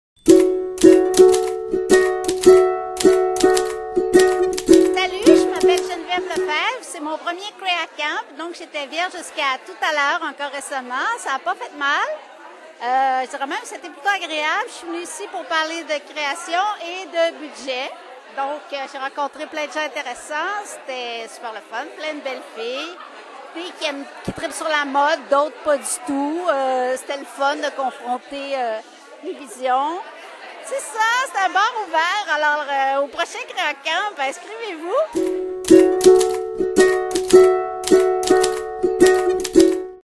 Geneviève Lefebvre aime Creacamp...
Geneviève Lefebvre scénariste et auteure parle de son expérience à Creacamp